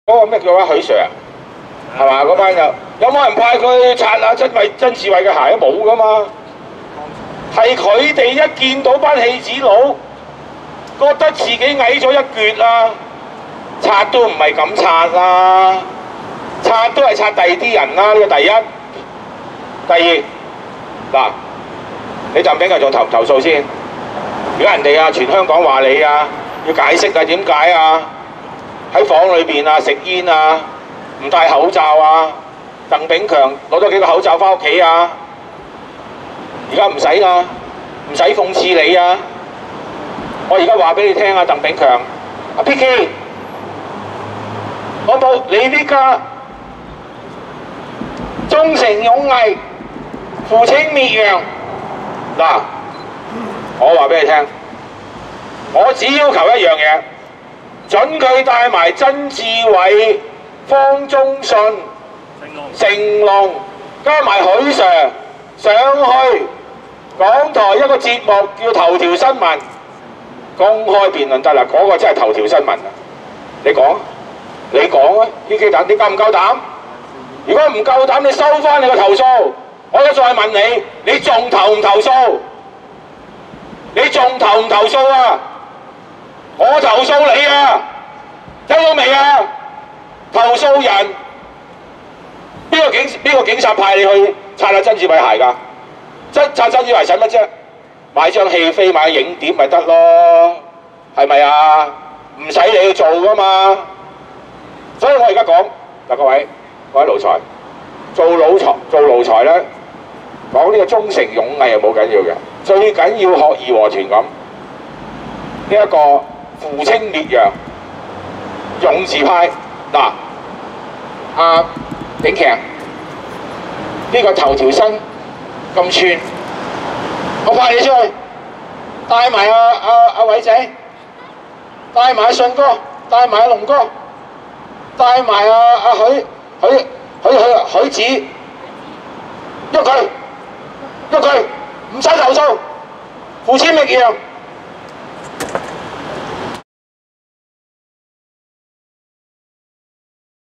亞鄧炳強，鞋並不是這樣擦的，為何要那樣矮化自己呢？
那個警察叫什麼名字？許SIR嗎？有人叫警察去擦曾志偉的鞋嗎？沒有呀！實情是那班警察一見到班戲子佬便覺得自己矮了一截呀！擦鞋都不是這樣擦吧！要擦都擦些有地位的人吧！

我現在問鄧炳強還會不會投訴？現在全香港人要他解釋為何在室內食煙？為何不帶口罩？鄧炳強到底拿了多少個口罩回家呢？現在已不需要頭條新聞來譏諷他，我要跟鄧炳強說一下「忠誠勇毅，扶清滅洋」。我現在就批淮他帶同曾志偉、方中信和成龍，再加上許SIR一同前往香港電台，出席一個他愛看的電視節目《頭條新聞》，來公開辯論一下，那一集就真的是頭條新聞。鄧炳強夠不夠膽應約呢？若他不夠膽赴約就收回他的投訴吧！
我再問鄧炳強一次：「鄧炳強，你還要投訴嗎？我現在要投訴你呀！你聽唔聽到？居然走去投訴人家，現在是警察派你去擦曾志偉鞋嗎？你要擦曾志偉鞋只要買張戲票、買隻影碟便成，為何你要親自去呃like呢？」

要做好奴才說忠誠勇毅是不打緊的，最重要是學義和團般「扶清滅洋」，勇字當頭。「鄧炳強，既然頭條新聞『咁寸』，我派你帶埋亞偉仔、帶埋亞信哥、帶埋亞龍哥，一齊去討伐佢，不用投訴了，直接去『移動佢』，扶清滅洋啦！』